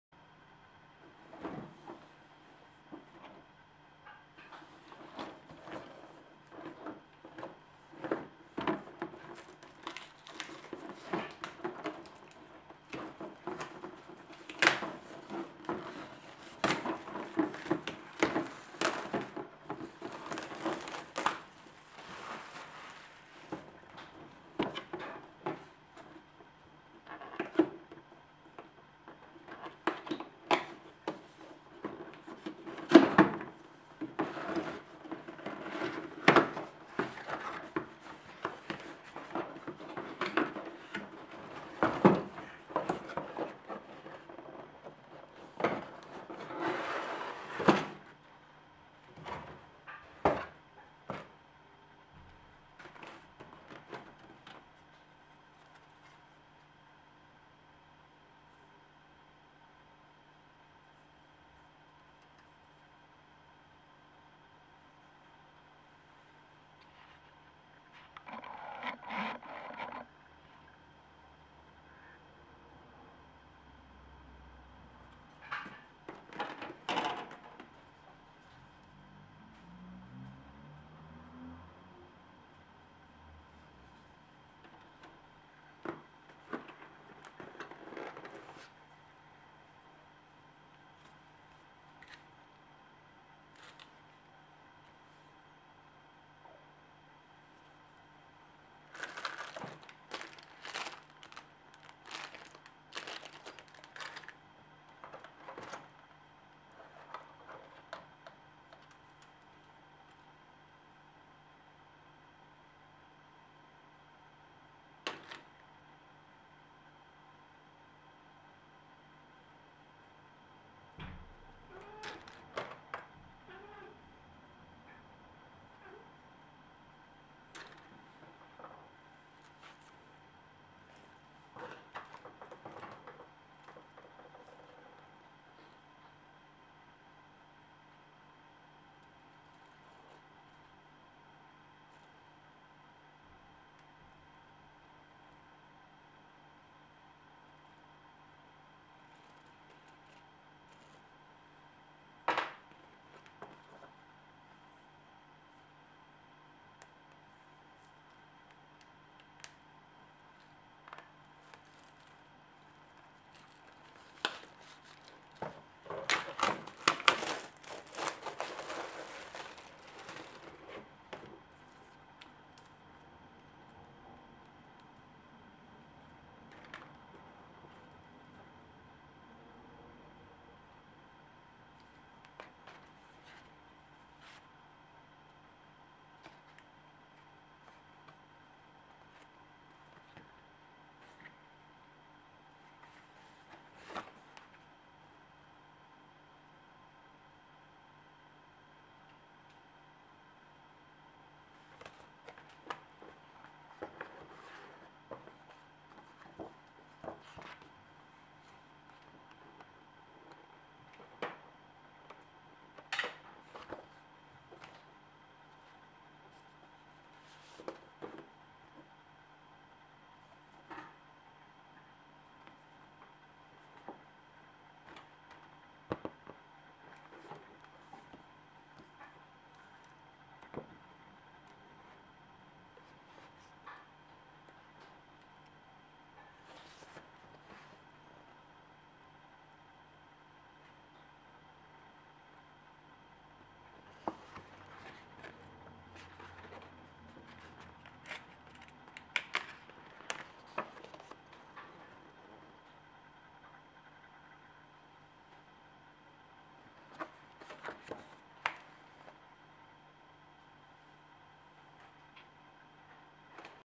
Warhammer 40k Kill Team Krogskull's Boyz unboxing.
Wonder what is in the box? This is it! Quick simple look at the content. Any questions about the cards or anything, fire away.